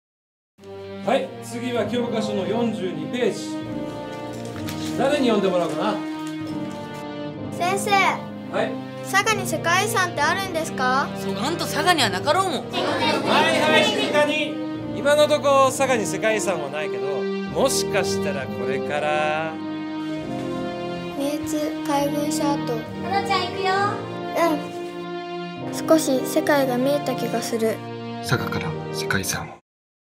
世界遺産登録推進テレビCM 【小学生篇】
「明治日本の産業革命遺産　九州・山口と関連地域」の構成資産の一つである佐賀市の『三重津海軍所跡』をPRするCM動画の第1弾です！